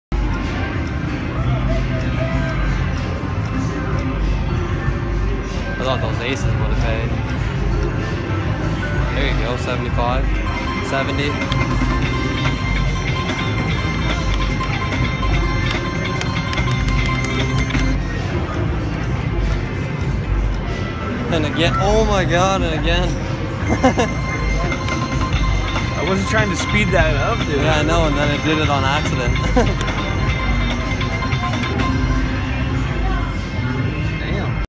Noahs Ark Slot Machine Live Win
Noah's ark slot
2 cent denom
6$ bet
Accidentally did another bet (not a big deal) and had another great win!

Sub if you'd like. Or not it's up to you!